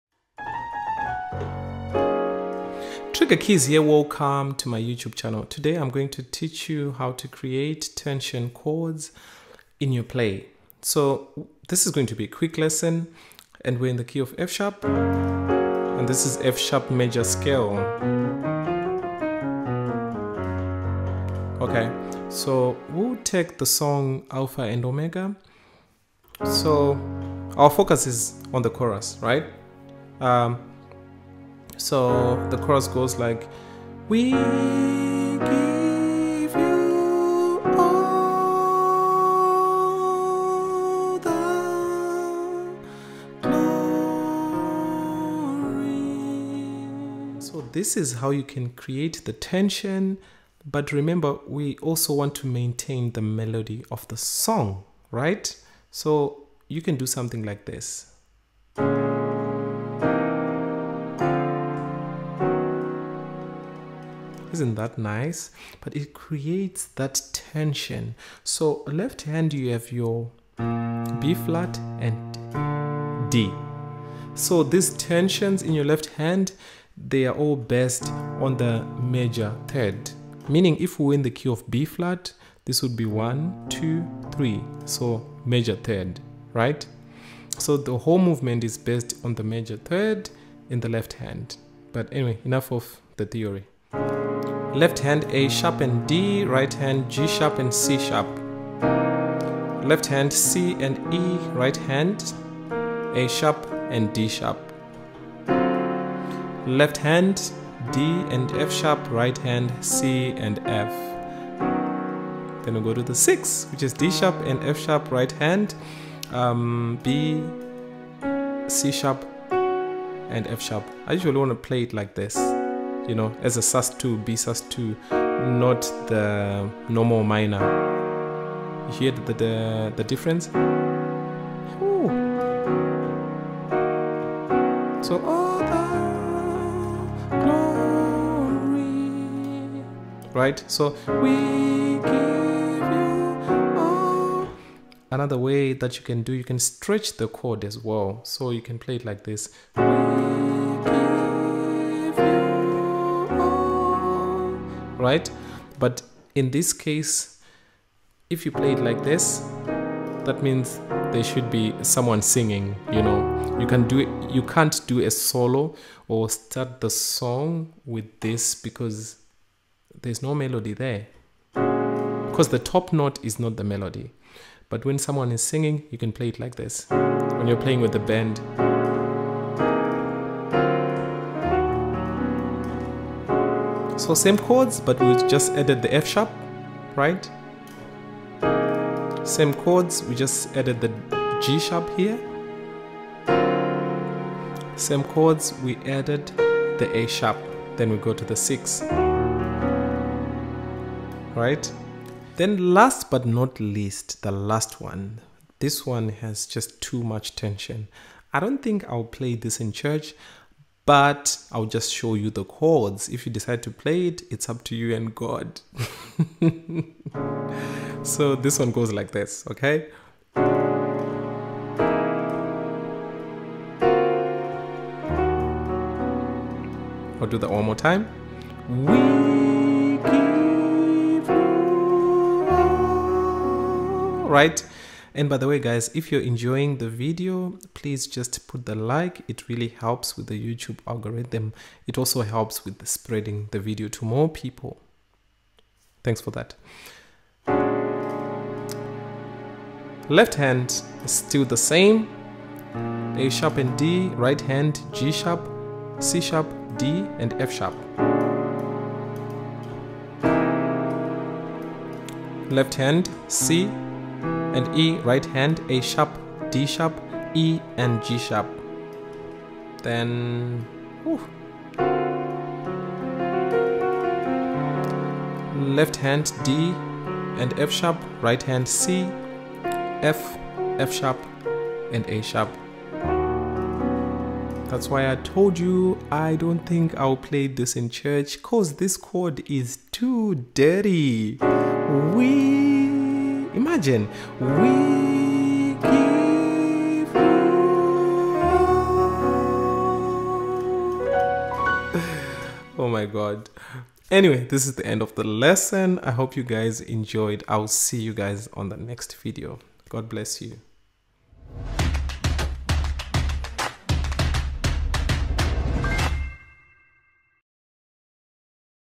How I create TENSION in my play
Hey guys I’m this video I’m showing you a simple movement you can use to create a bit of tension in your play.